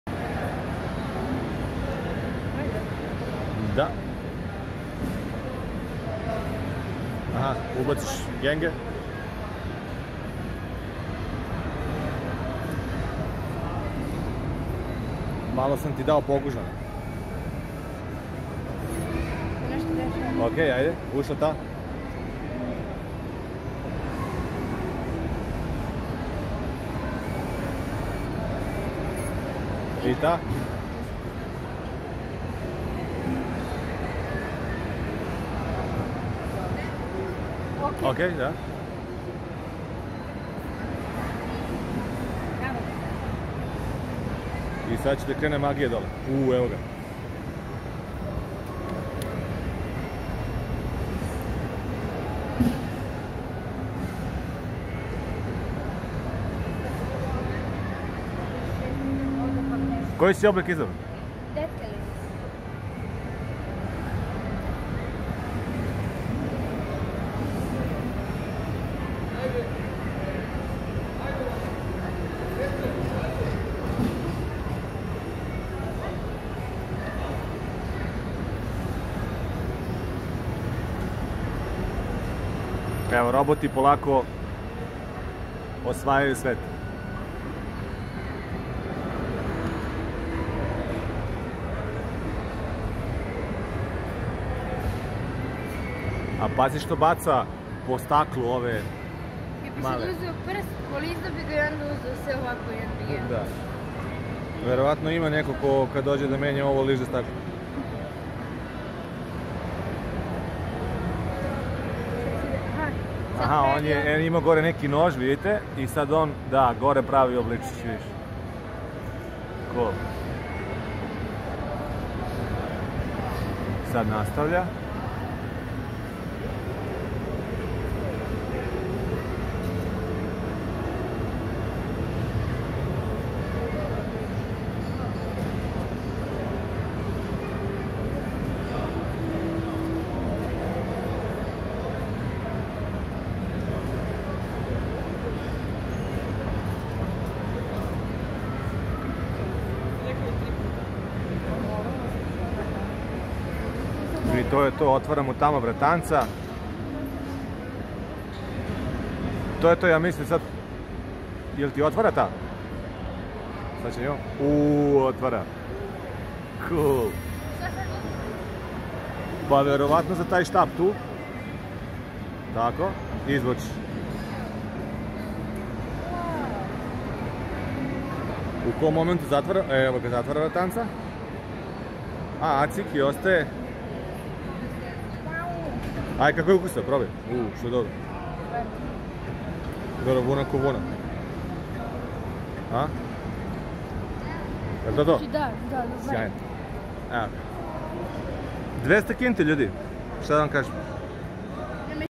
Cotton Candy, robot za secernu vunu,pravi oblike koje god pozelite.
Robot pravi secernu vunu,oblikuje je,pravi kako god pozelite.

The robot makes cotton candy, shapes it, makes it however you want.